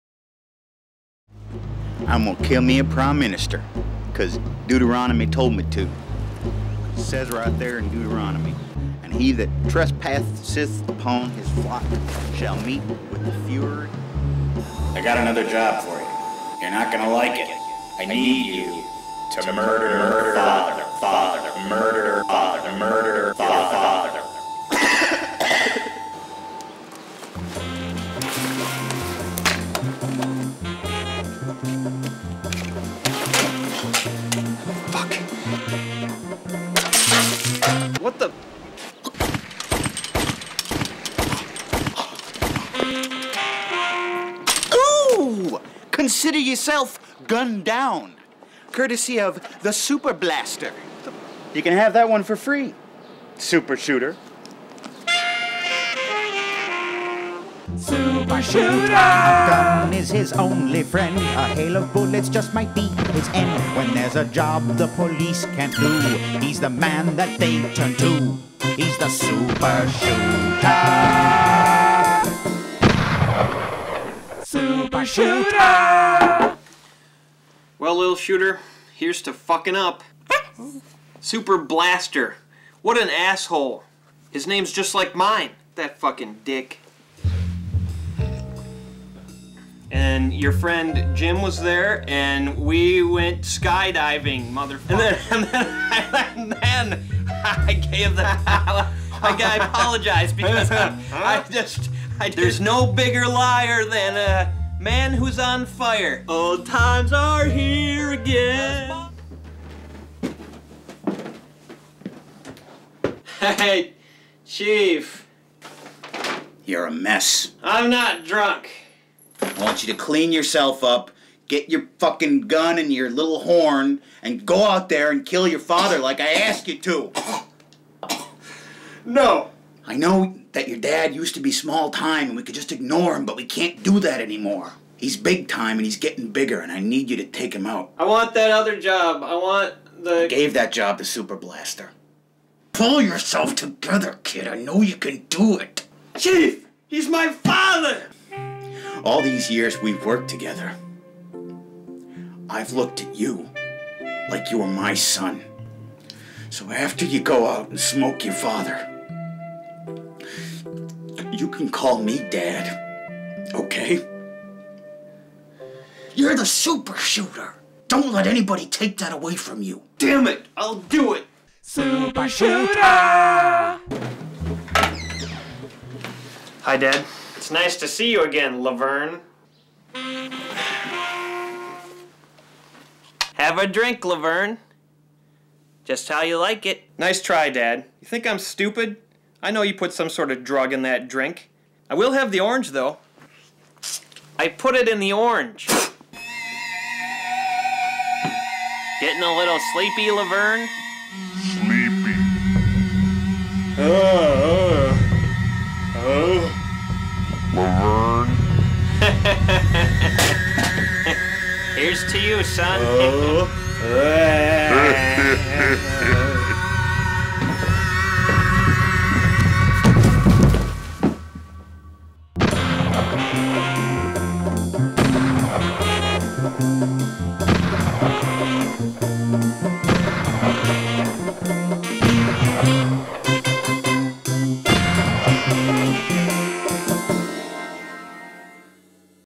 Super Shooter 2 "Drunk For Hire" (remastered)
Super Shooter struggles with two shitty problems--his competitor Super Blaster, and his hardest job ever, taking out his own father.